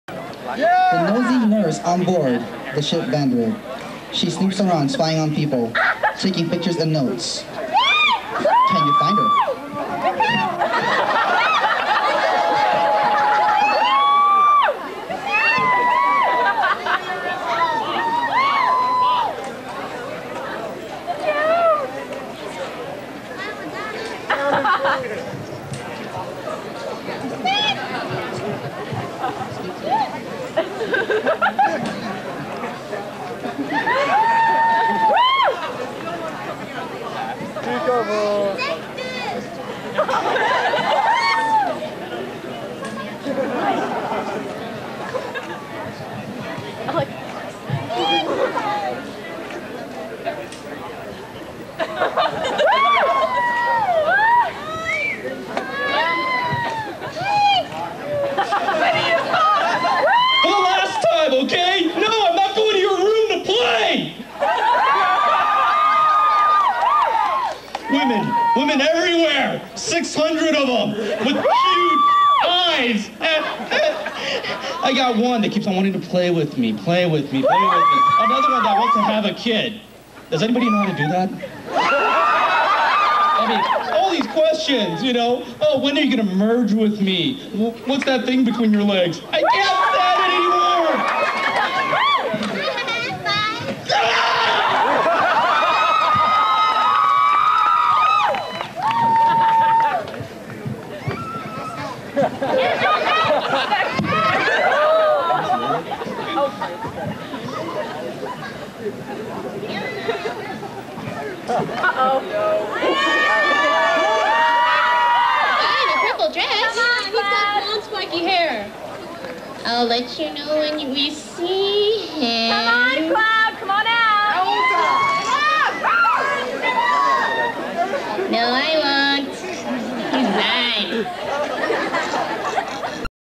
Ani-Magic 2003 - #10 Vandread
Video of Entry 10, cosplay skit of Vandread, performed at Ani-Magic 2003. (Permission granted)
(Won Mike Toole's Judges Award "Best Vandread")

For other videos, be sure to check my favorites and please